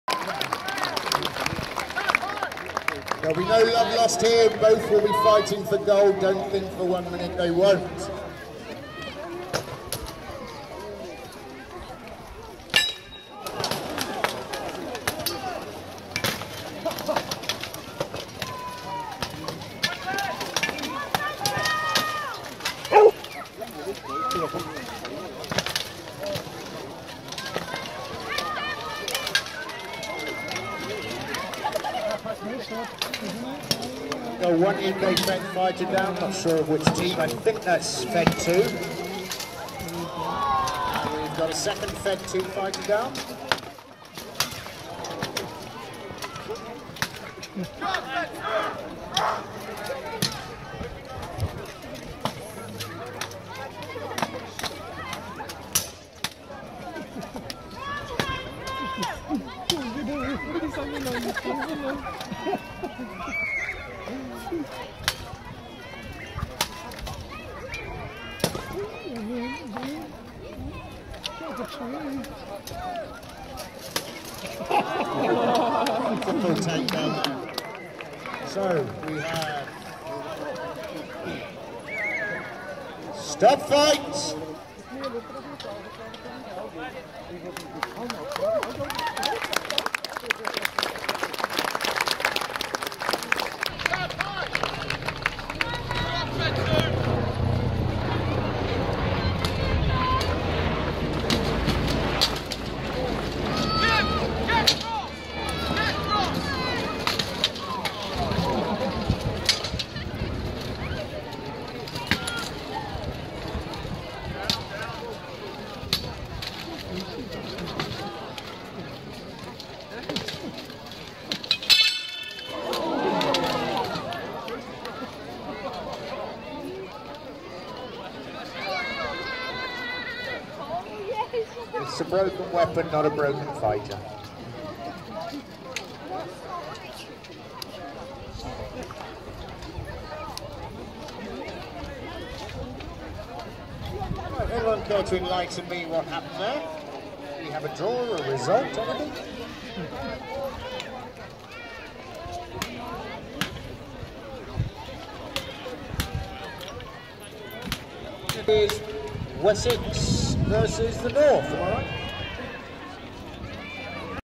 Battle 4. UK Federation 1 vs UK Federation 2
ROUND 1
5v5
Richard -1td! / Edd +1td*
4v5
Dave -1td! / Rob +1td*
3v5
Jim B. ±1td / Barney ±1td
2v4
James H. -1td(vs2) /  Rob +0.8td*, Daniel +0.2td
1v4
Glen -1td(vs4) / Edd +0.25td, Daniel +0.25td, Rob +0.25td, James DF +0.25td
0v4

ROUND 2
Ashley -1td! / Edd +1td*
4v5
Jim S. -1td! fell when was trying attack
3v5
Michael +1td* / James DF -1td!
3v4
Glen -1td(vs2) / Barney +0.75td, Edd +0.25td
2v4
Michael -1td / Barney +0.5td, Rob +0.5td
Jim B. -1td! / Brian +1td*
0v4